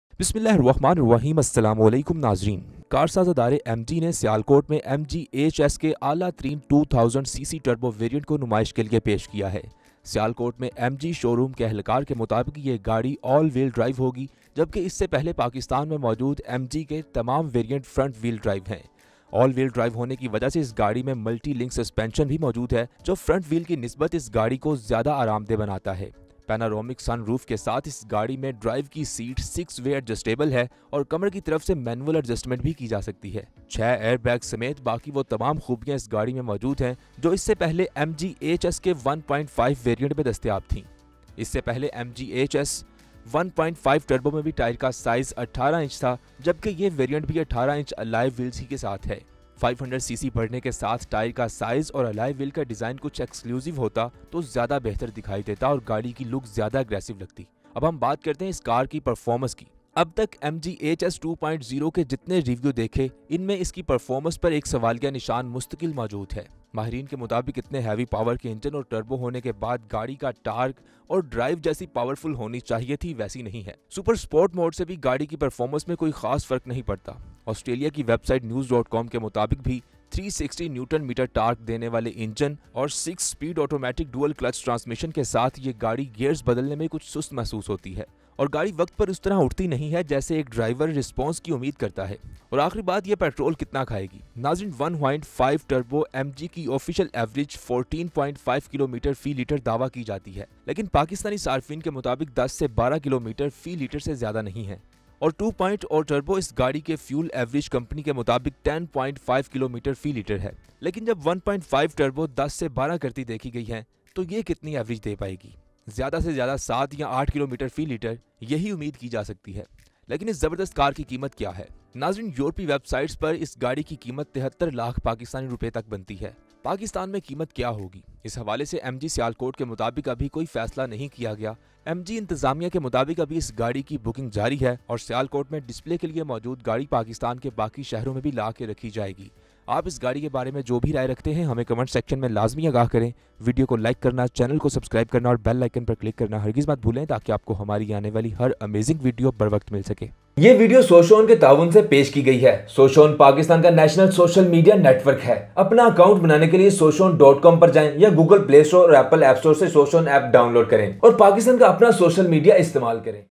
MG HS 1.5L Turbo Review in Pakistan. Watch Features and Price || 3W Coffee
Morris Garages (MG) Motors, a Chinese-owned British automotive company, has launched its first car in Pakistan. The MG HS is a subcompact crossover SUV with a sporty exterior and stylish interior. Internationally, the car comes in four variants, but only one is available in Pakistan. MG HS is a premium SUV competing with a number of other new entrants in the SUV segment of the local Pakistani market.

MG Motors acquired the greenfield status under the Automotive Development Policy 2016-20. Javed Afridi, the CEO of Haier Pakistan, partnered with the company to set up a vehicle assembly plant in Pakistan. MG Pakistan, then, debuted in the local market with MG HS in November 2020. The company did not host a launch event and casually started selling the SUV in the market. Still, this first MG car in Pakistan received an overwhelming response from the public.